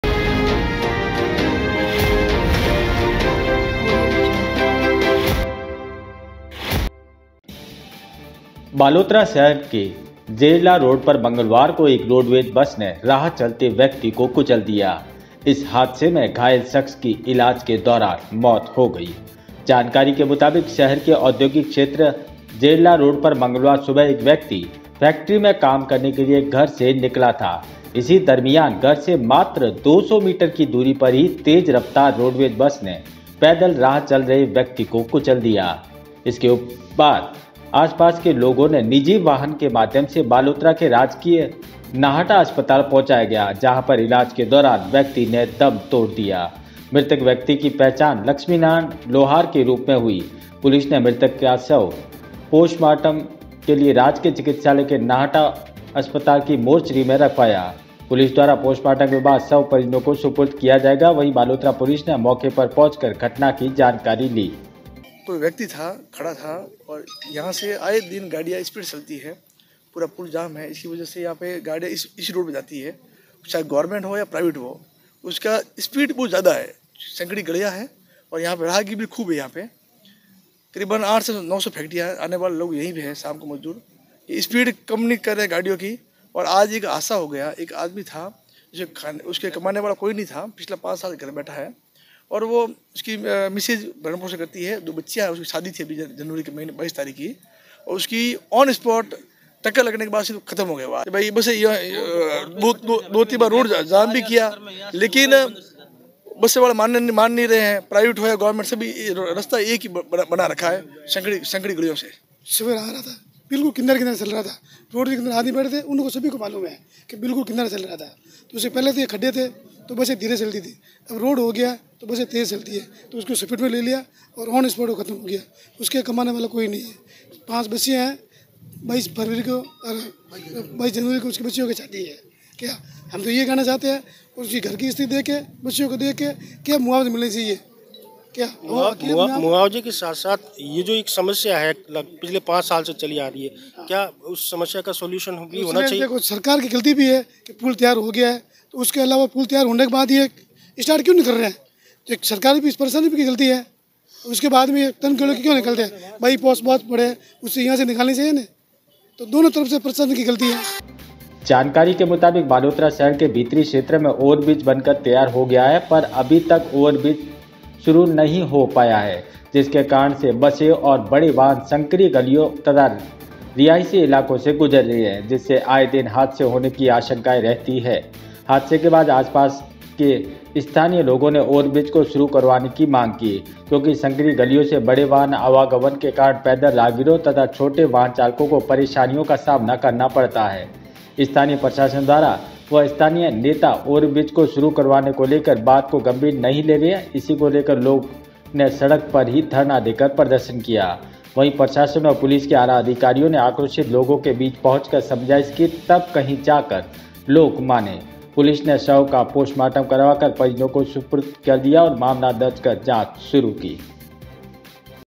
फैक्ट्री में काम करने जा रहे व्यक्ति को रोडवेज बस ने कुचला, लोगों ने सड़क जाम कर किया धरना प्रदर्शन
फैक्ट्री में काम करने जा रहे व्यक्ति को रोडवेज बस ने कुचला, स्थानीय लोगों ने सड़क जाम कर किया धरना प्रदर्शन